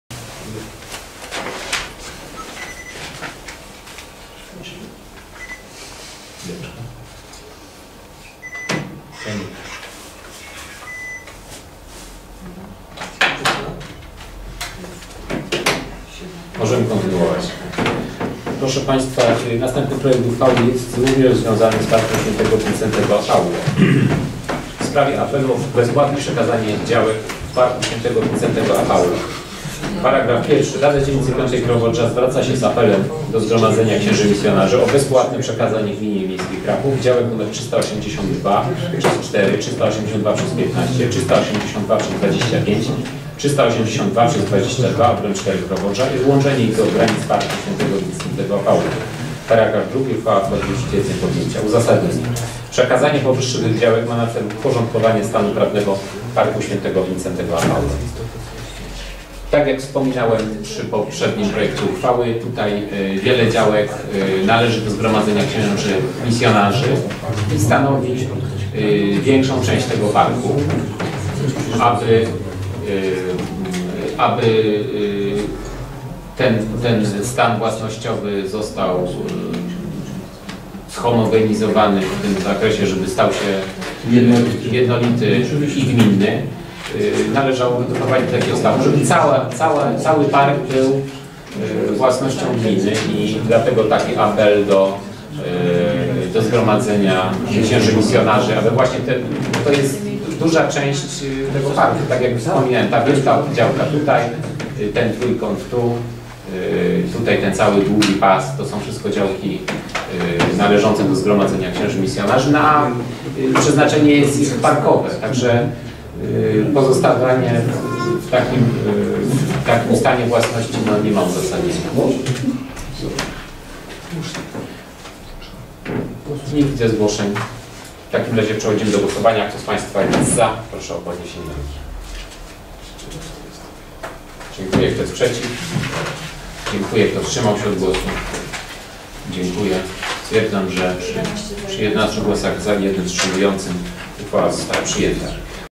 RD_V_11-06-2013_13 bezpłatne przekazanie działek przez Zgromadzenie Misjonarzy Park Wincentego a Pa
Nagranie z XXXIII sesji Rady Dzielnicy V Krowodrza z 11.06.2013- cz.13- omawiane tematy i głosowania:

-ws. uchwały nr XXXIII/309/2013 ws. apelu o bezpłatne przekazanie działek w Parku św. Wincentego a'Paulo